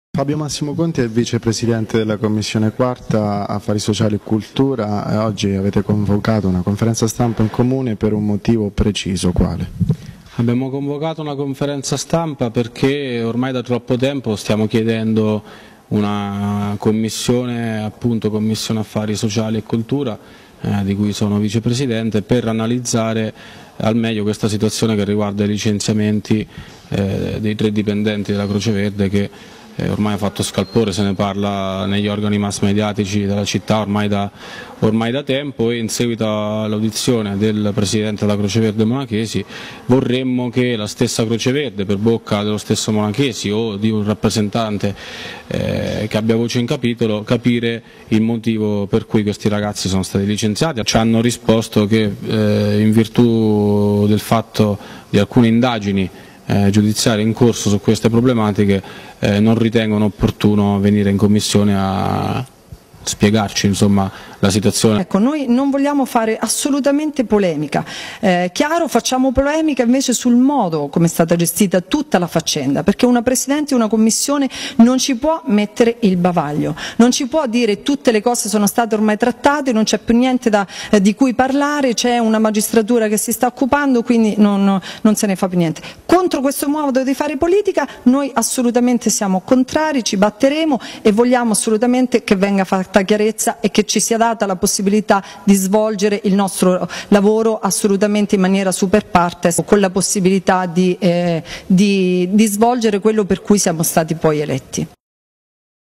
MACERATA. NIENTE COMMISSIONE SULLA CROCE VERDE: LA MINORANZA ALZA LA VOCE
Macerata. 9 agosto 2013. Anna Menghi, Francesca D'Alessandro, Fabio Massimo Conti e Marco Guzzini, componenti della commissione IV Affari Sociali e Cultura lanciano il grido d'allarme sul licenziamento dei tre dipendenti della Croce Verde. La richiesta di convocazione della commissione infatti è stata bocciata dalla Presidente Gabriella Ciarlantini con motivazioni che hanno lasciato fortemente perplessi gli esponenti dell'opposizione. Servizio di Claudio Ricci.